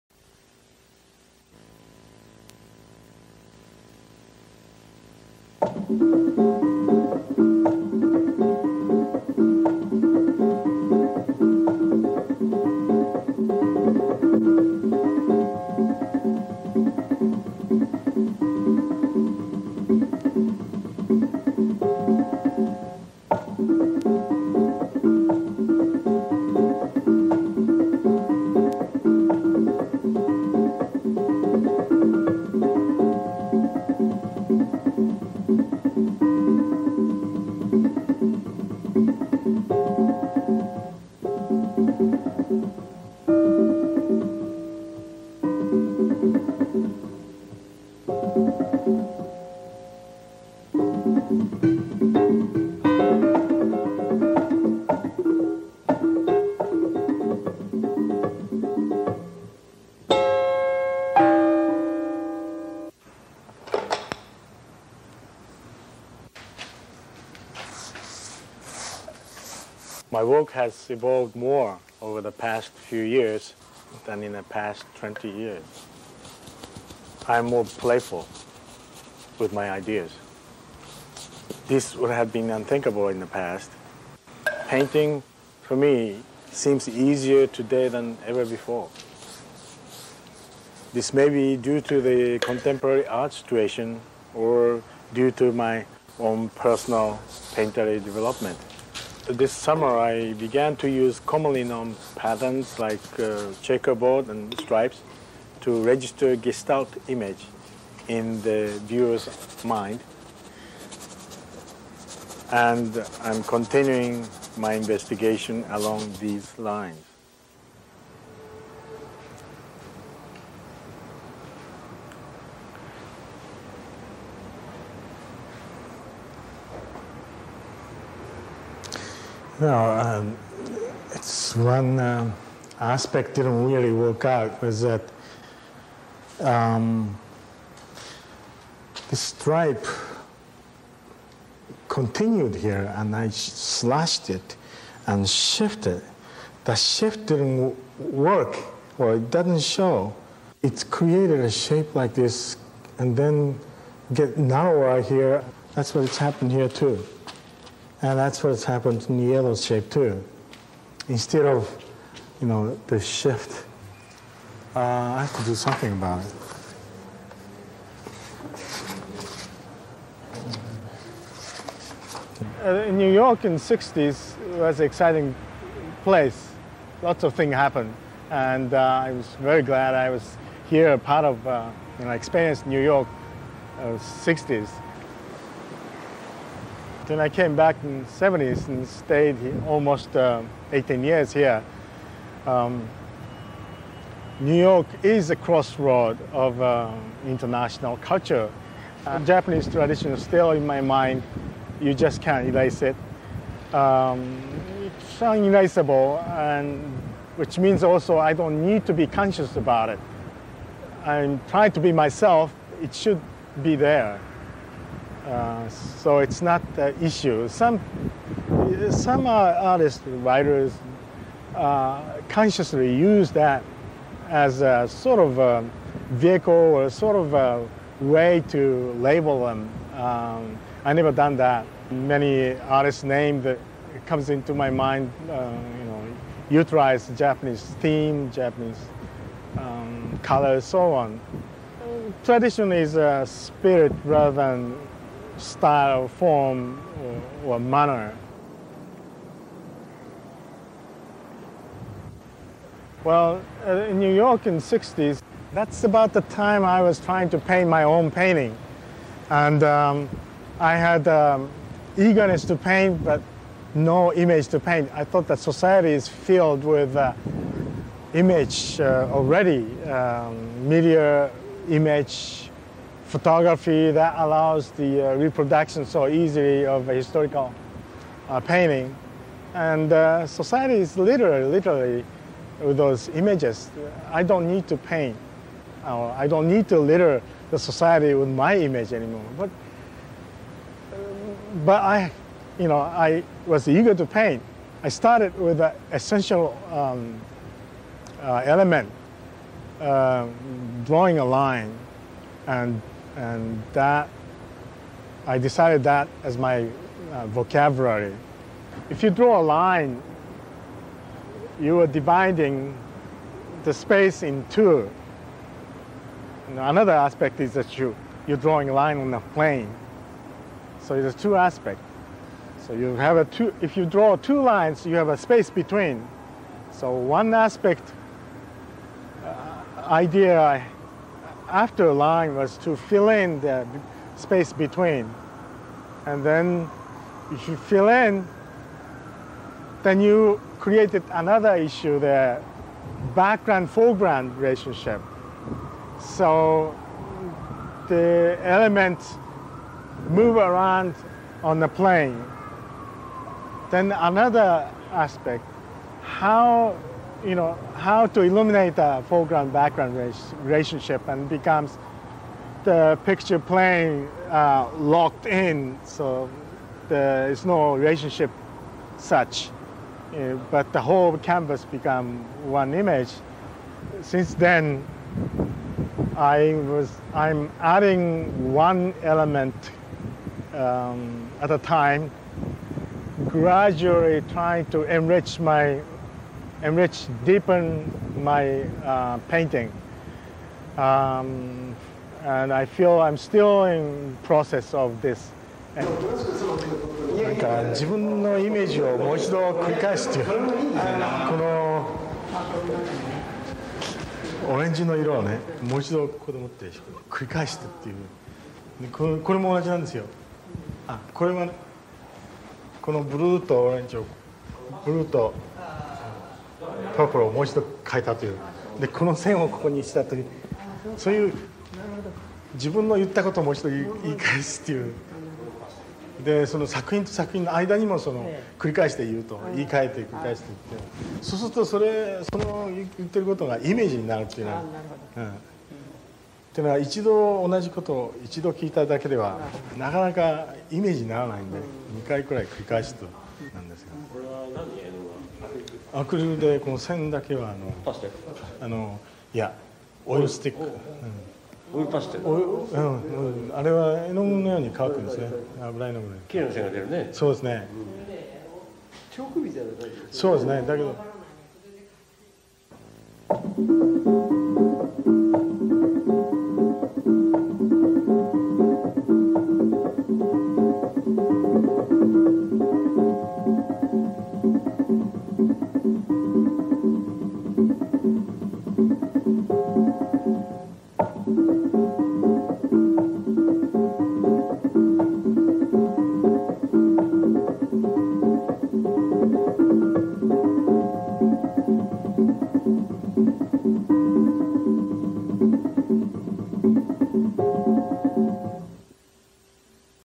Hitoshi Nakazato Interview at his studio in NY, 1989
Hitoshi Nakazato (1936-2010) first went to the US in 1962. He returned to Tama Art University, his alma mater, to teach for several years, but he spent the rest of his career as an artist in New York City.

When Nakazato arrived in the US, the influence of Abstract Expressionism was waning. Counterculture sparked artistic movements like Neo-Dada and Pop Art which incorporated images from popular culture; fragments of daily life tumultuously invaded the sacrosanct realm of painting. These art movements were a formal objection to the notion of high art secluded in the ivory tower. Anti-Vietnam War protests were taking place on university campuses throughout the US. After graduating from the University of Pennsylvania, Nakazato returned to Japan through Europe and encountered the civil unrest in Paris in May 1968. It was a time of rebellion against the establishment. Japan was also experiencing revolutionary fervor as student protestors blockaded classrooms at the Tama Art University. Student-led organizations forced faculty members, including Nakazato, to confess their complicity to the establishment. Living through such a time, Nakazato confronted the contradictions of an artist living in contemporary society. He even considered stopping painting but deliberately chose to continue working in the medium, simultaneously striving to break free from the realm of painting. With this premise, he began his own painting revolution rooted in his determined thought processes and tenacious physical endurance.

Printmaking provided a valuable means for Nakazato’s abstract images. He first encountered cutting-edge printmaking methods in the US, and mastering multi-color single-plate printing propelled the development of his abstract art.

Camera: Dieter Froese
Second Camera: Doug Eisenstark
Post Production: Dekart Video
Creative Consultant: Sumiko Takeda
Subtitles: Jin Nakazato